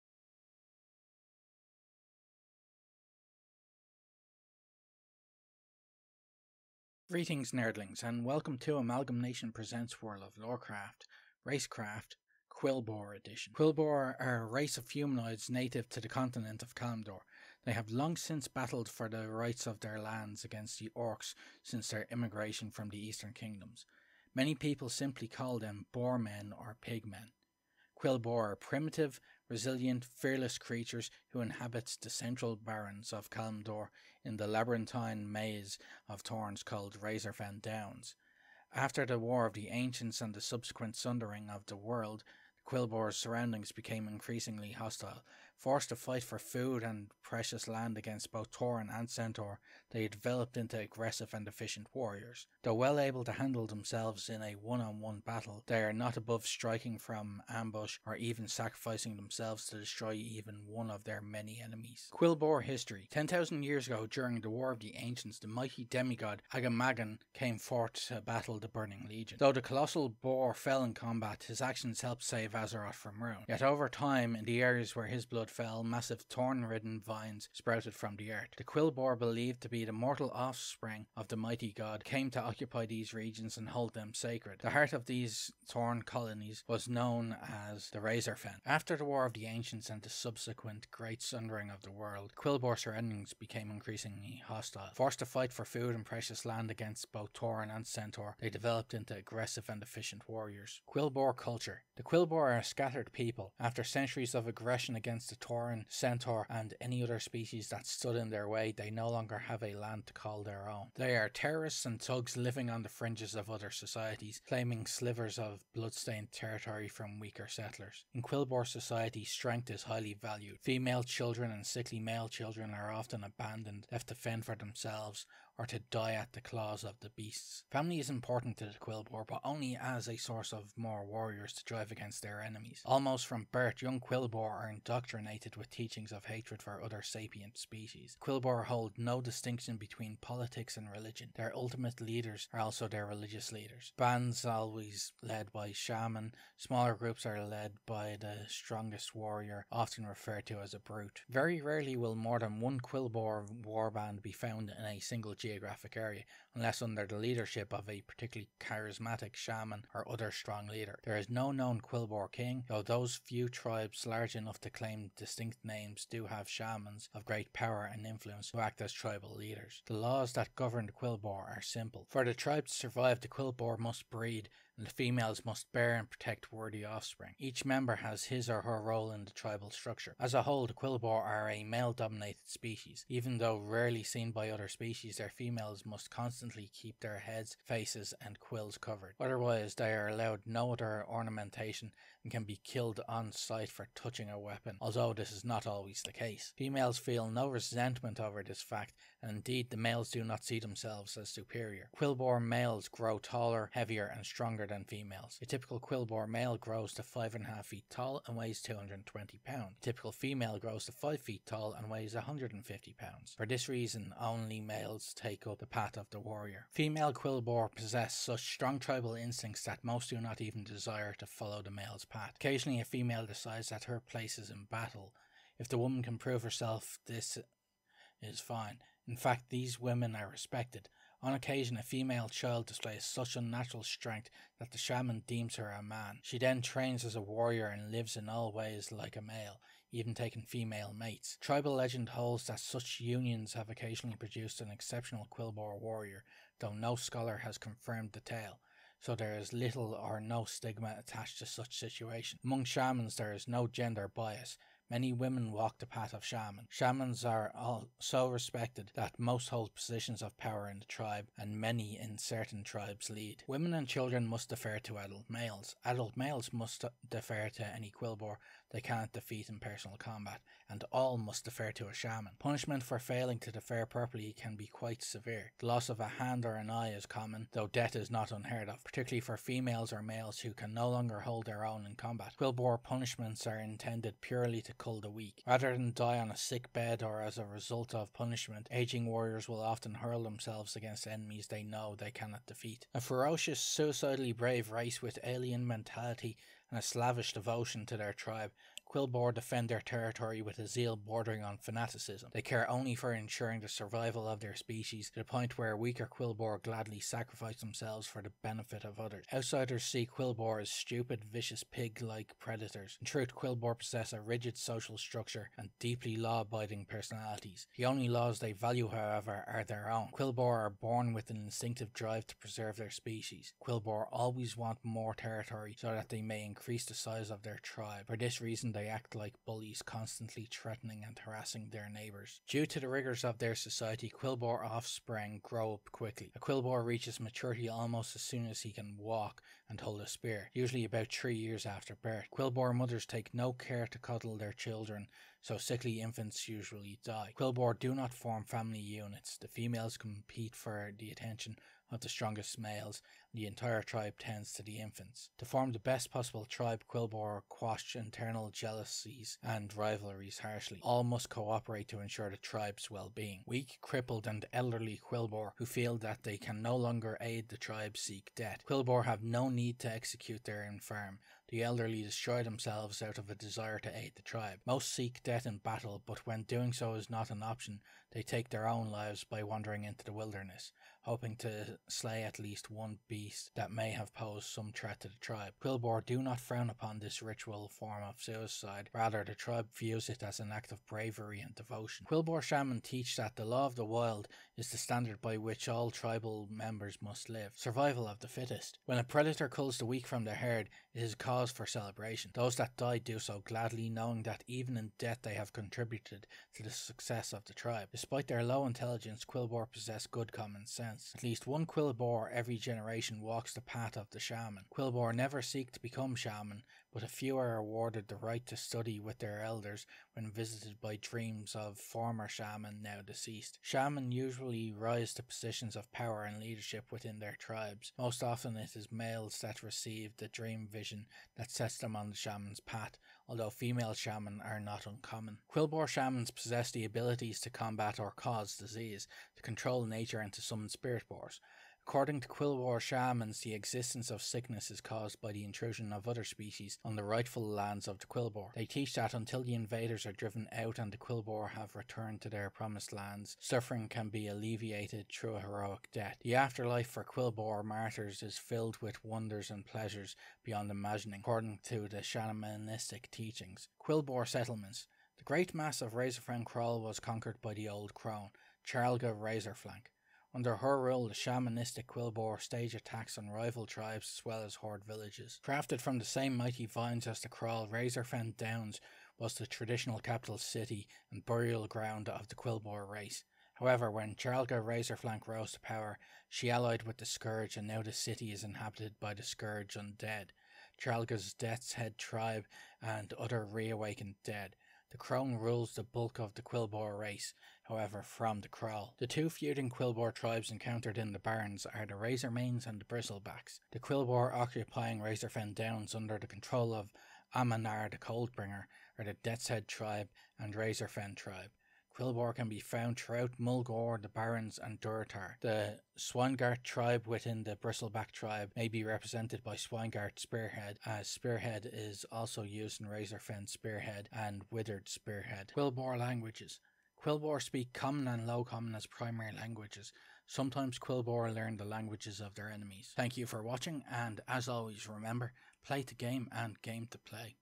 World of Lorecraft: Racecraft: Quilboar Edition
The lore and information behind the brutal Quilboars of Kalimdor